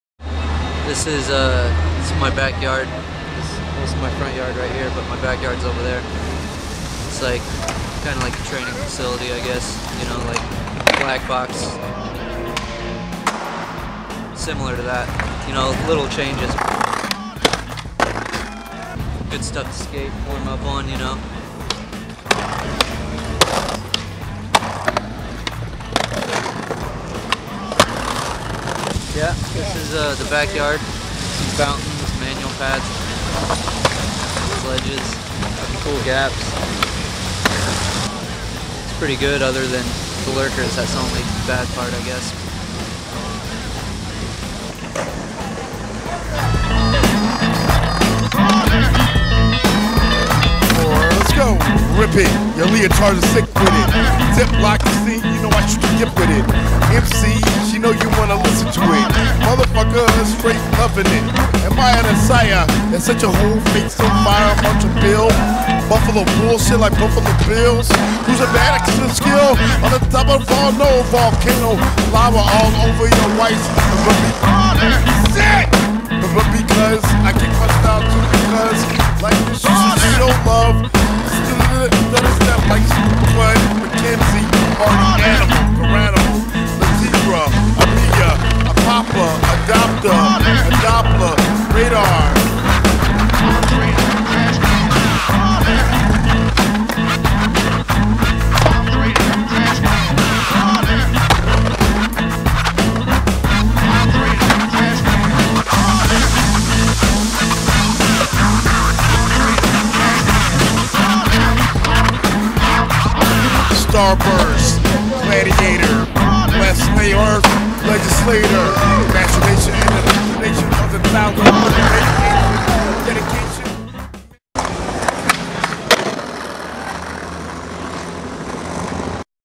Peter Ramondetta's BACKYARD (not really)
Dennis Busenitz in some real skateboards video. Amazing!!!